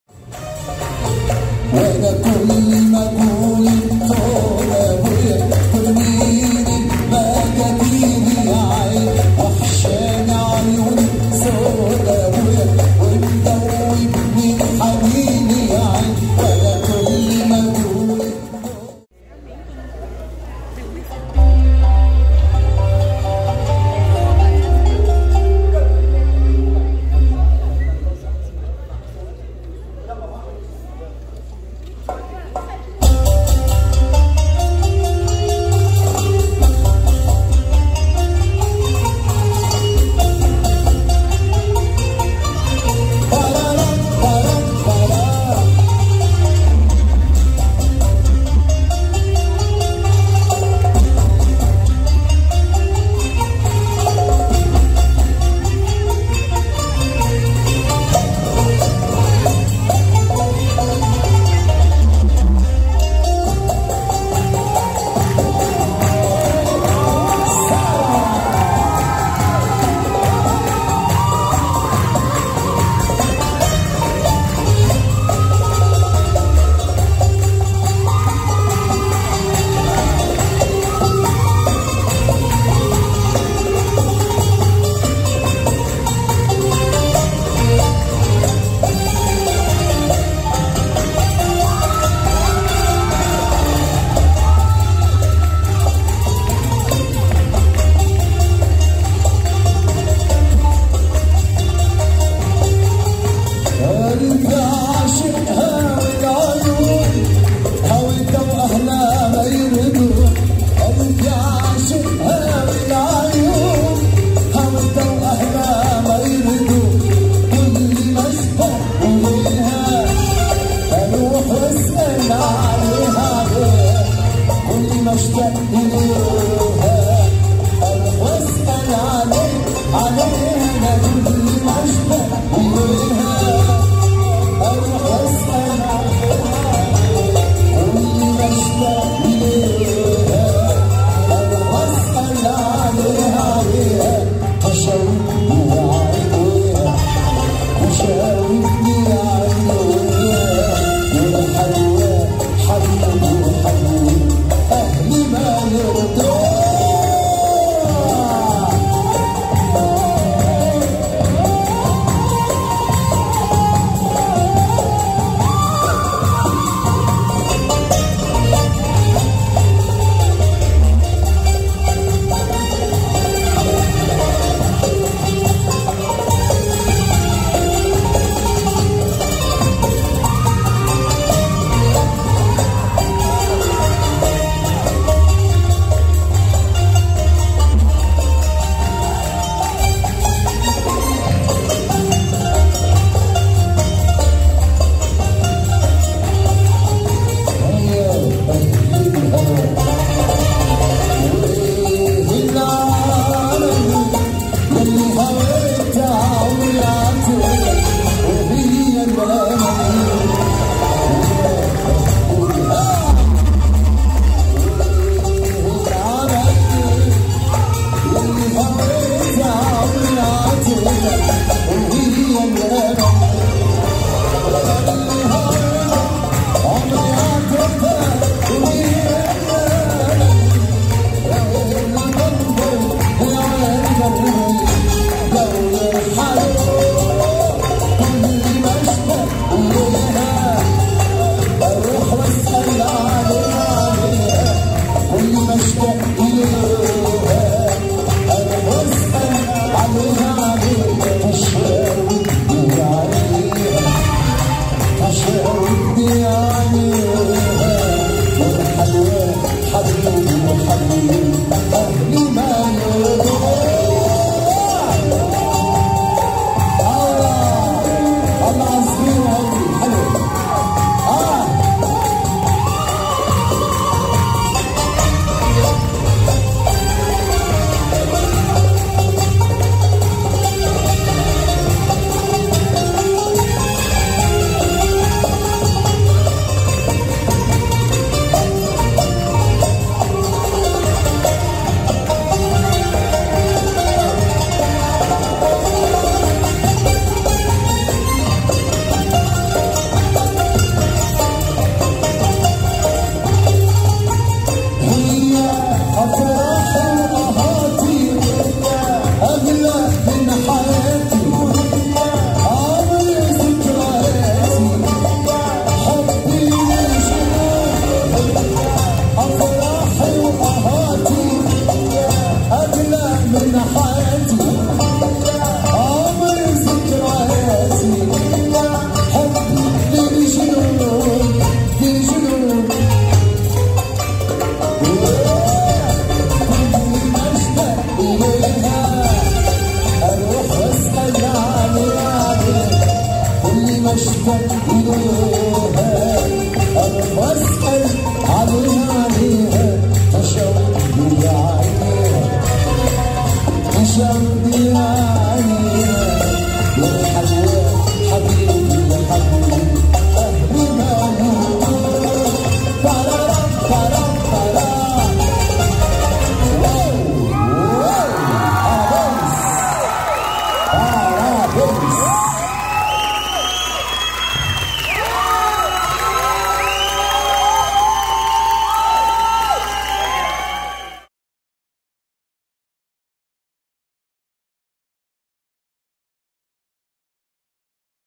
Festival Raqs Rubyy-Alika Sarah e Thais 2022-Albi
O Raqs Rubyy é um Festival de Danças Árabes, com apresentações das bailarinas do Studio Sarah Rubyy, artistas profissionais como a bailarina Bruna Nassif, o especialista em folclore Teo Versiani, a professora Sarah Rubyy, grupos de artistas Árabes renomados no Brasil, como o Nasser Mohamed e o cantor Tony Mouzayek e banda. @tonymouzayekoficial1560 @tatagalhardoo @studiosarahrubyy